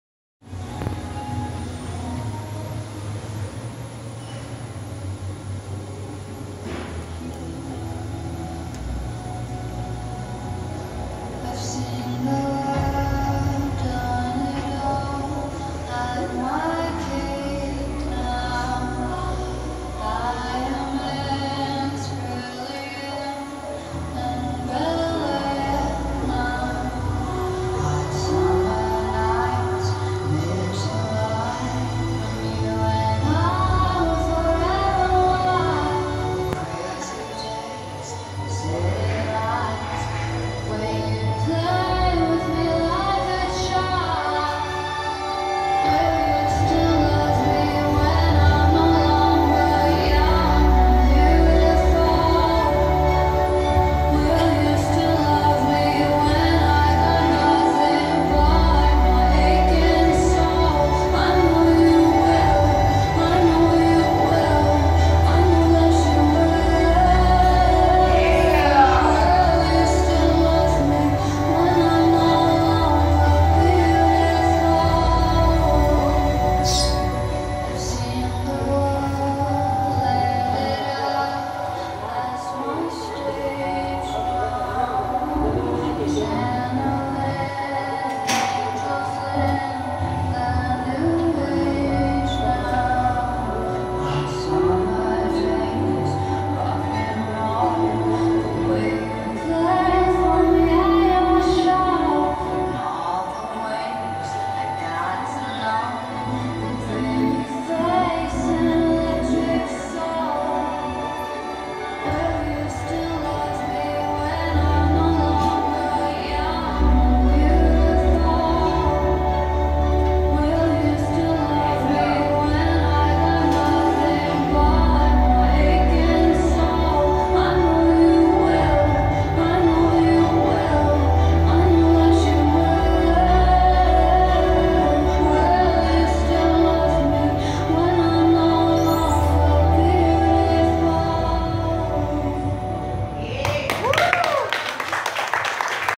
Aplicación Venezuela Pole Championship 2017 - Gabriela Navarro
Presentacion realizada el Viernes 7 de abril en el Gimnasio Tennis shop